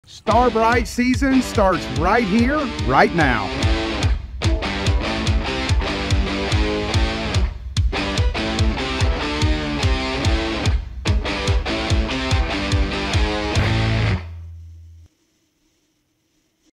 Star brite Season Kickoff with Bobby Lane | Unleashing the Spirit of Summer
StarbriteSeason is HERE! 💥 Join our Team Star brite Angler, Bobby Lane, in showcasing the magic of summer and the power of Star brite products. Star brite Season starts RIGHT HERE, RIGHT NOW! 🎣🚤